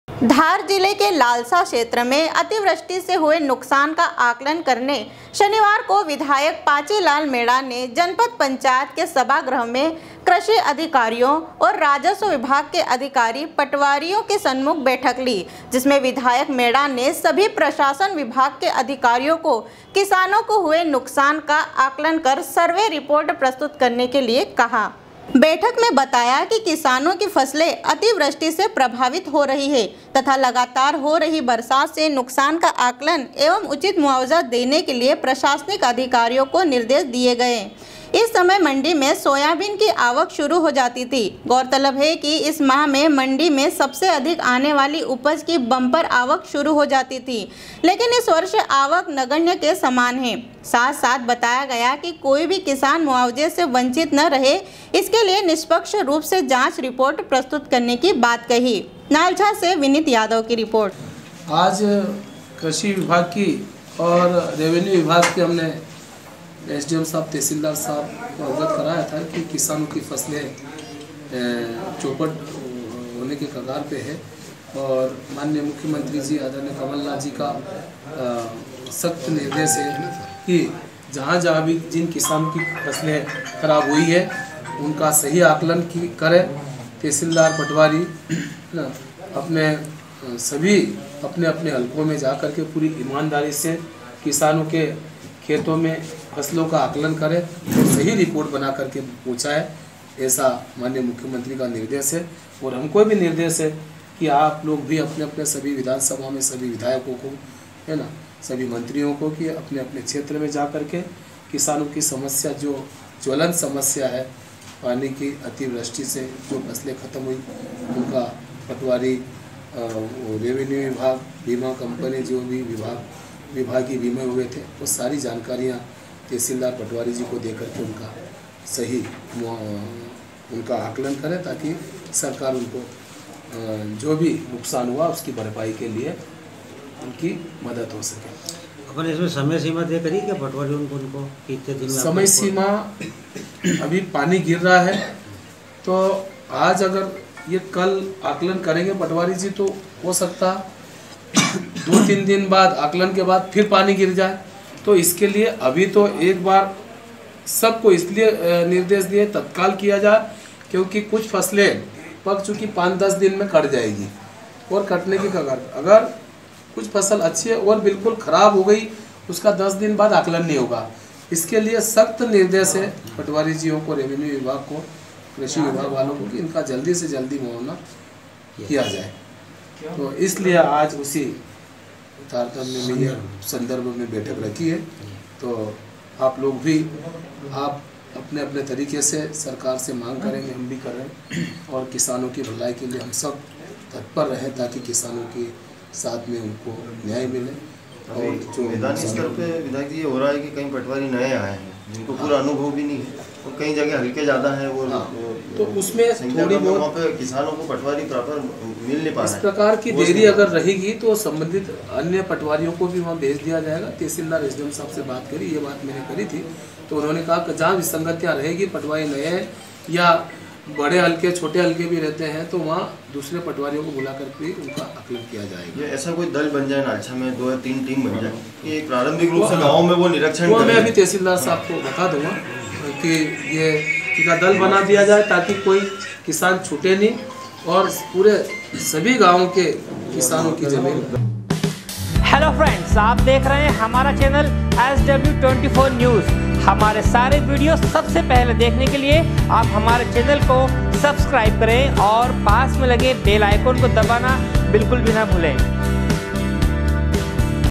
sw24news nalcha, 30.09.2019
-धार जिले के लालसा क्षेत्र में अतिवृष्टि से हुए नुकसान का आकलन करने शनिवार को विधायक पाची लाल मेड़ा ने जनपद पंचायत के सभागृह में में कृषि अधिकारियों और एवं राजस्व विभाग के अधिकारी पटवारियों के सम्मुख बैठक लीsw24news